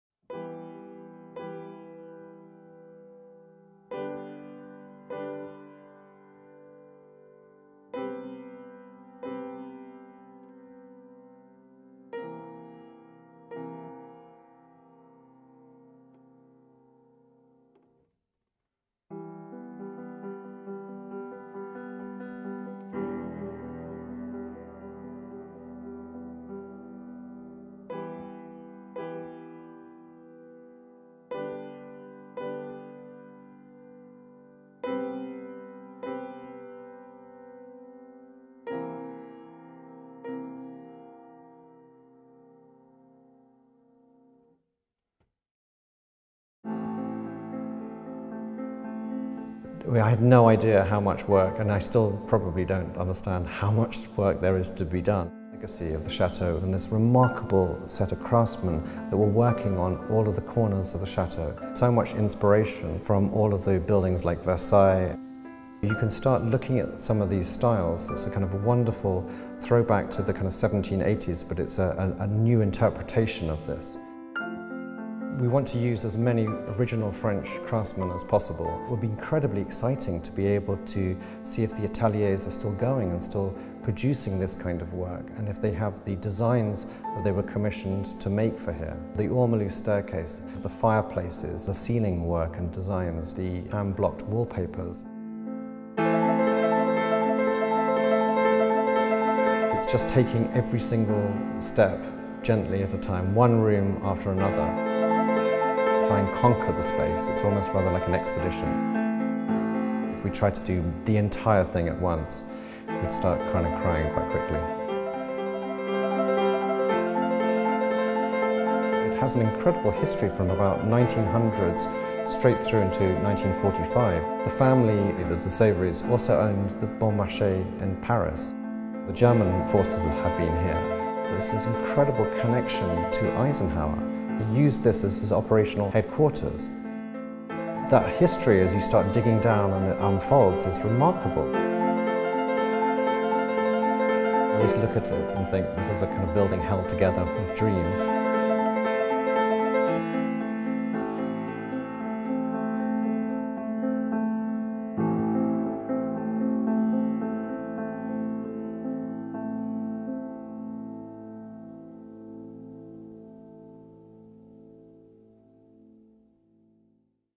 'Restoration Chateau' with Tim Gosling & Steve Holmes in Normandy, France
Follow Tim and Steve's restoration journey as they peel back the layers of time & craftsmanship. Built in 1820 this Normandy gem had the good fortune to be redesigned by the designer of The Ritz, Monsieur Georges Farcy for the Bon Marché family in 1910. The chateau has been untouched since the retreating German army removed the heating system in the 1940’s. Eisenhower moved into the chateau when he was appointed supreme commander of Operation Overlord, the Allied invasion of north-western Europe. Restoration Chateau is a classical Parisian design, with 57 rooms, 22 ensuite bedrooms (revolutionary for its time), 8 staterooms and 27 acres. Over the coming months please join us on a journey of discovery as we piece together history and restore this classic building back to its former glory.